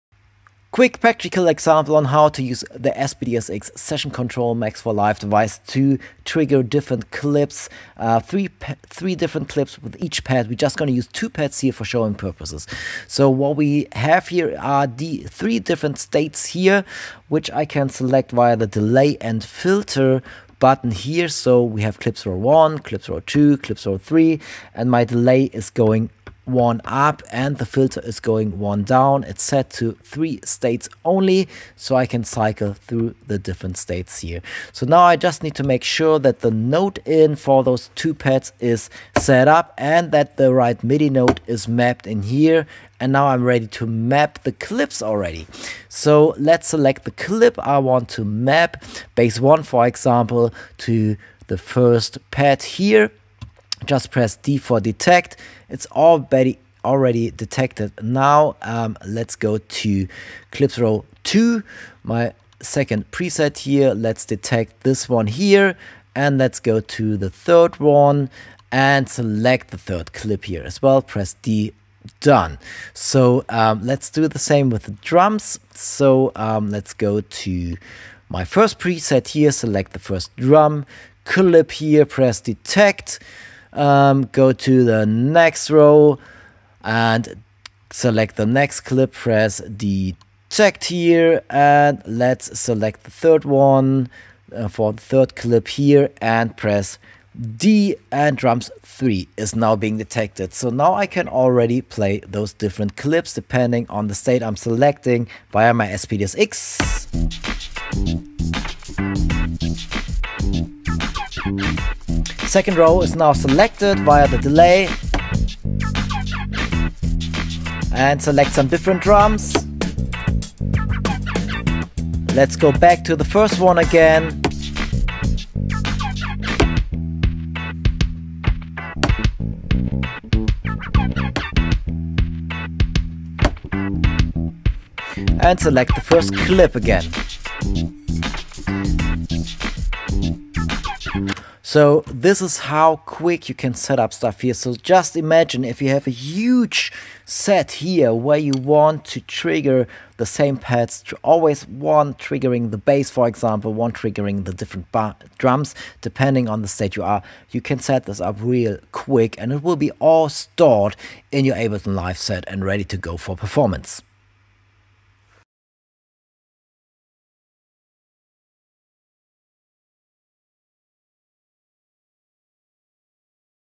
Trigger Clips in Session View in Ableton Live via Roland SPD-SX with Max for Live
CLIPS Quick
0:00 Intro
0:15 3 Presets / States for the Session Control device
0:38 Mapping Note In Pitch
0:50 Setting up Clips to be triggered
1:43 Triggering Clips in Ableton Live
2:21 Outro Check out more Max for Live devices here: